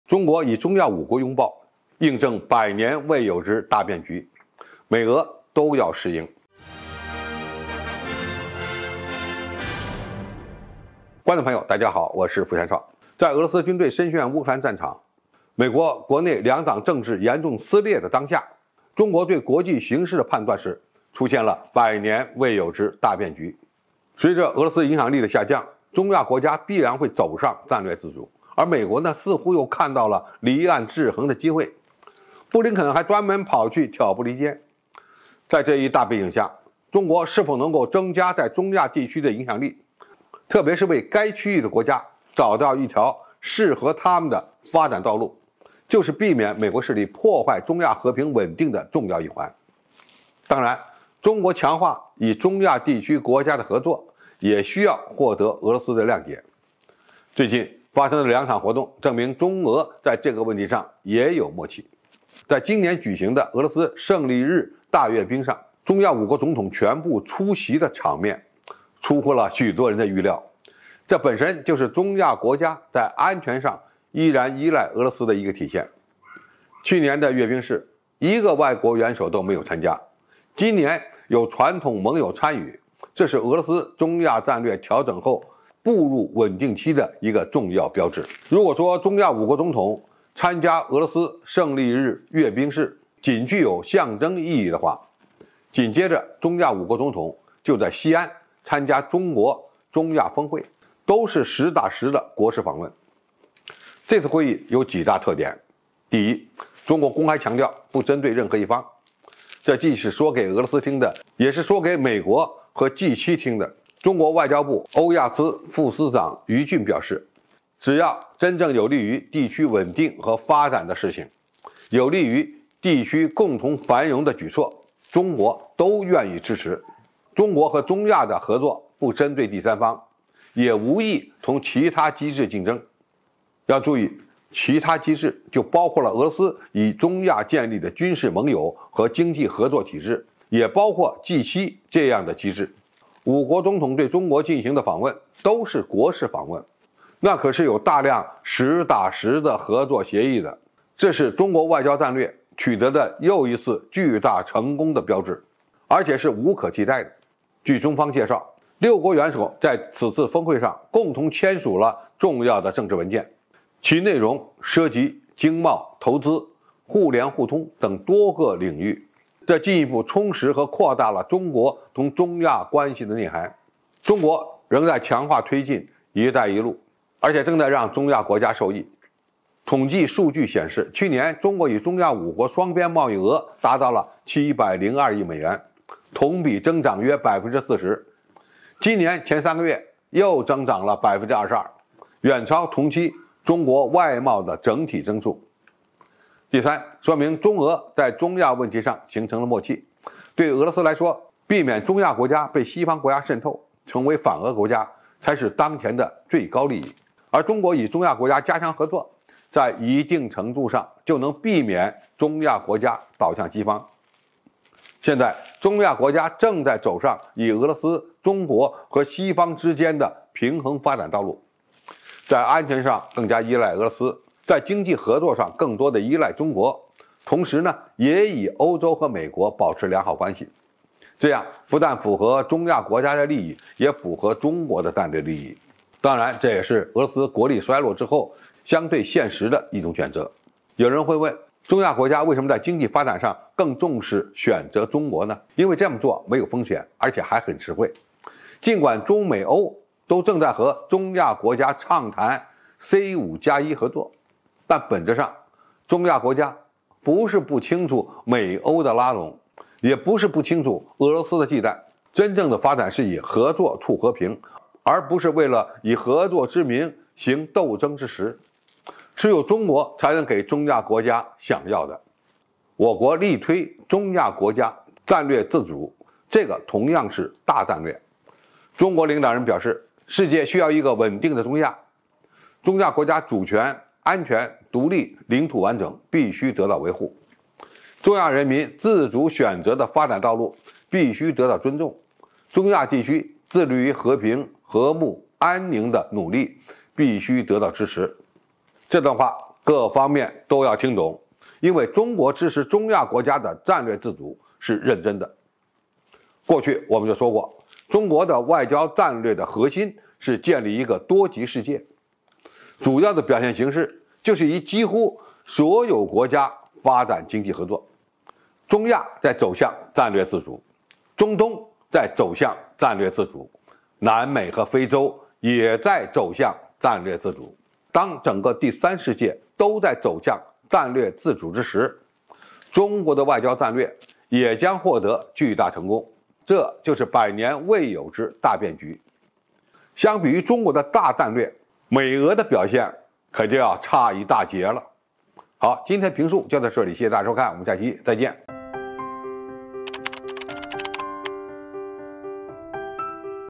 中國與中亞5國擁抱，印證“百年未有之大變局”，美俄都要適應【傅前哨】
在俄羅斯軍隊深陷烏克蘭的當下，中國對國際局勢的判斷是出現了“百年未有之大變局”
中國與中亞5國擁抱，印證“百年未有之大變局”，美俄都要適應
矚望雲霄 傅前哨 本頻道製作的所有影片都是為了向觀眾傳遞有趣的社會時事新聞，絕對符合平台的使用政策。